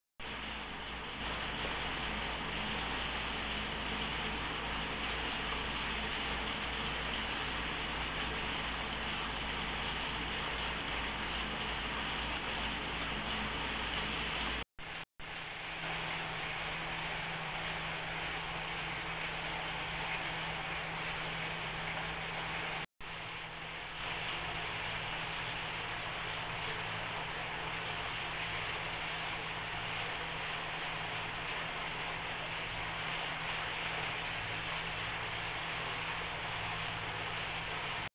Indoor Pondless Waterfall Installation in Greenwood, Indiana
Ambiance Gardens installs pondless waterfalls indoors in and around Greendwood, Indiana.